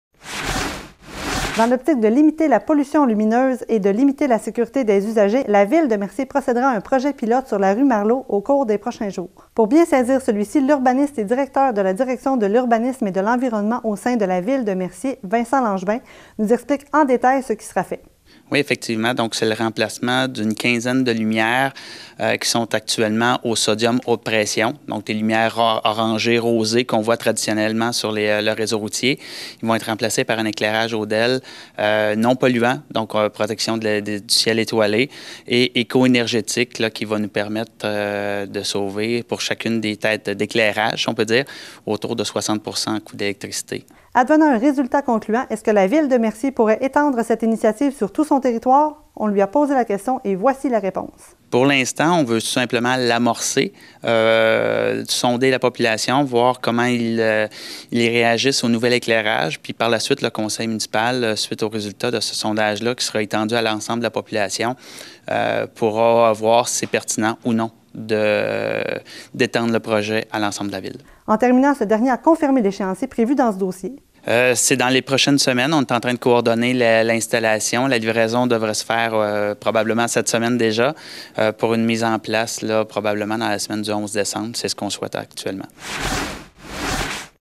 Projet-pilote Mercier
La Ville de Mercier entamera, au cours des prochains jours, un projet-pilote qui prendra place sur la rue Marleau.